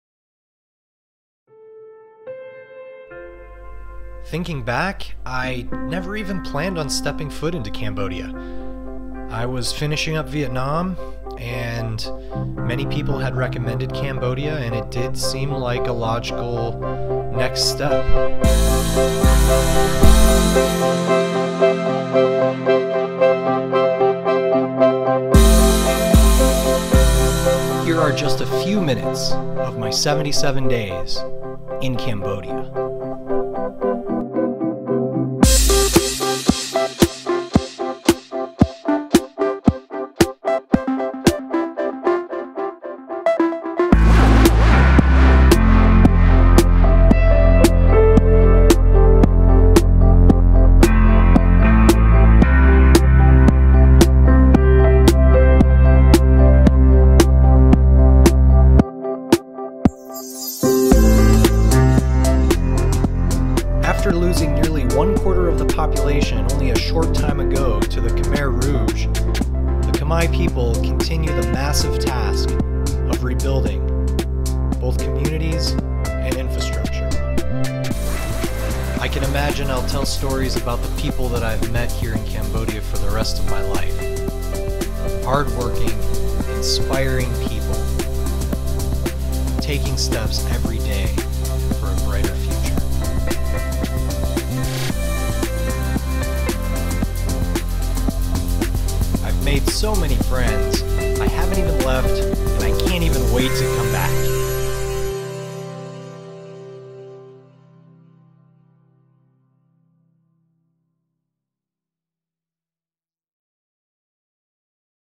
Cambodia || Adventure Everyday
I've had such a grand Adventure in Cambodia.  I've met so many inspiring people.  Its funny to think that this place has made such an impression on me and I never even planned the visit.  If you are interested in renting a motorcycle Victory Motorbike has highly maintained machines at honest prices (link below). I would highly recommend visiting the Cardamon Mountains as well. Its unfortunate but the jungles are disappearing.  Jungle Cross provides safe and exciting Motorcycle tours through the mountains (link below).  Also check out the video I made about a family I lived with in the Jungle for almost two weeks.